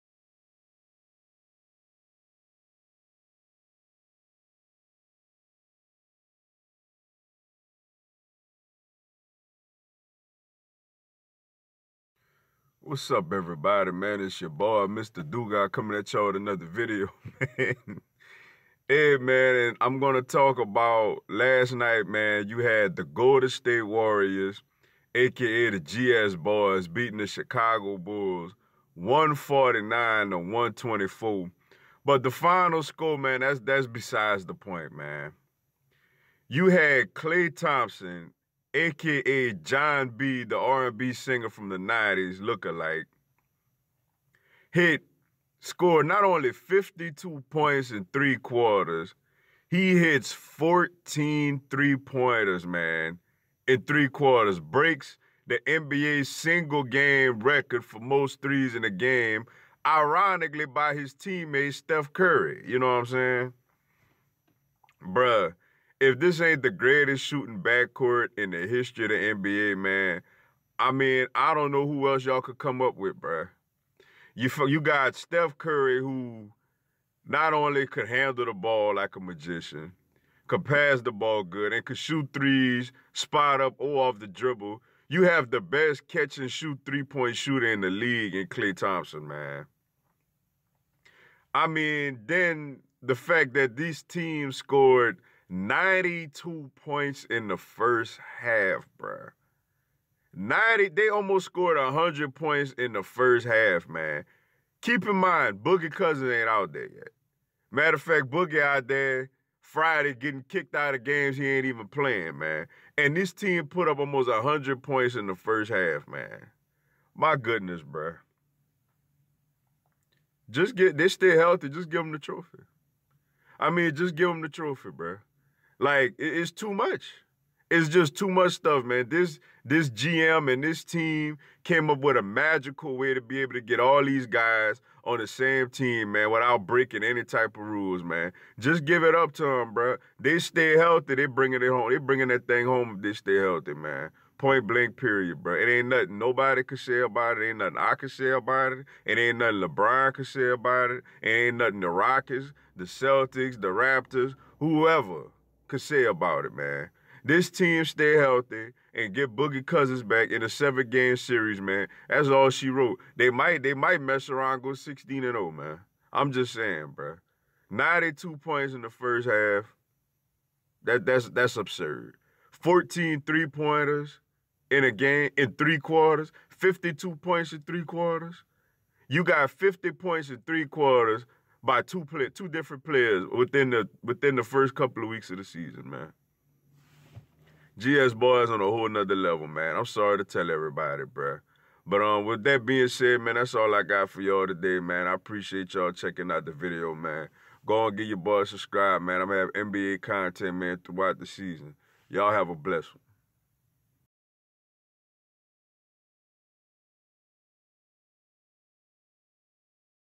Reaction to Golden State Warriors 149 Chicago Bulls 124. 92 points 1st half, Klay Thompson 14 3's😂
NBA, Western Conference, Eastern Conference, Golden State Warriors, Chicago Bulls, Klay Thompson, Steph Curry,DeMarcus Cousins, Lebron James